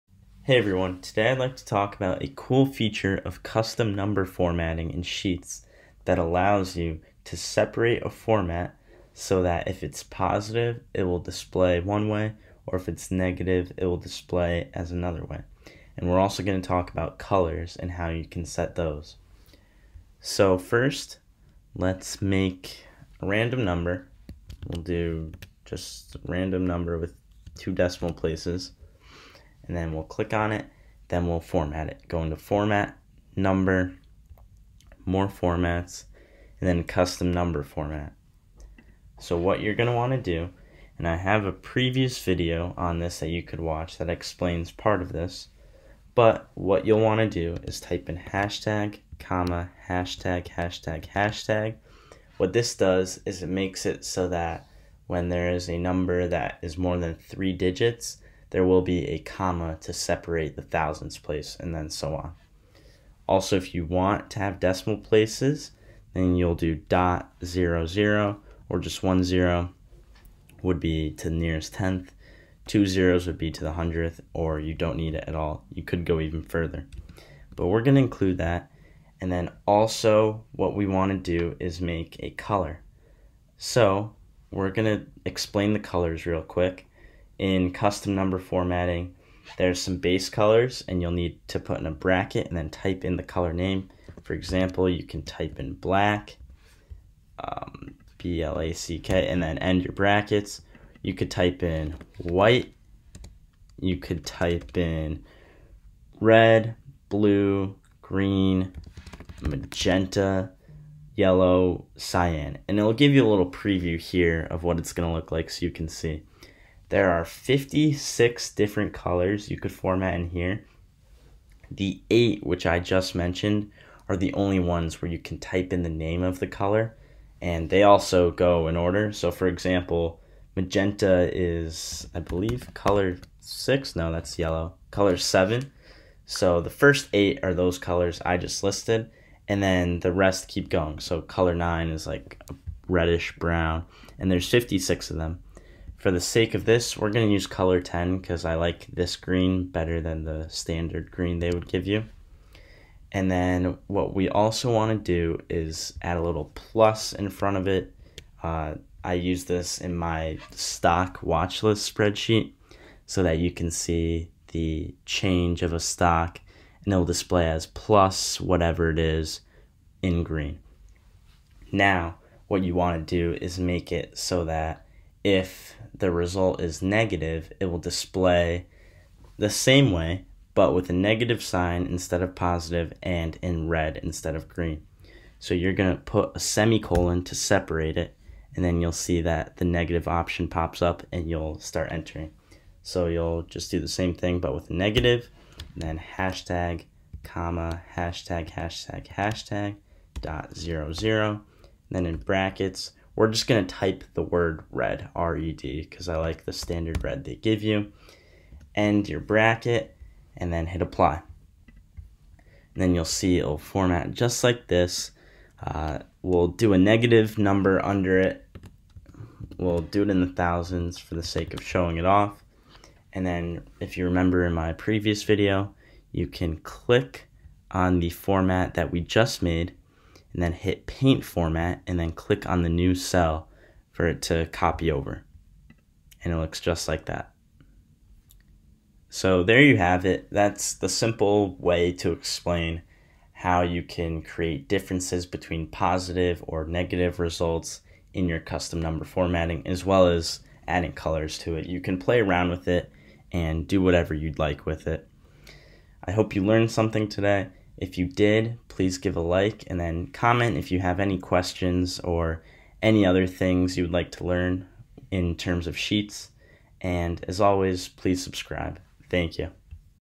FORMAT POSITIVE AND NEGATIVE NUMBERS WITH CUSTOM NUMBER FORMATTING
With Custom Number Formatting you can alter the way values appear in a cell in many ways. In this video we will talk about some of the basic syntax of custom number formatting and how you can use it to alter font color and add other features fro positive and negative numbers.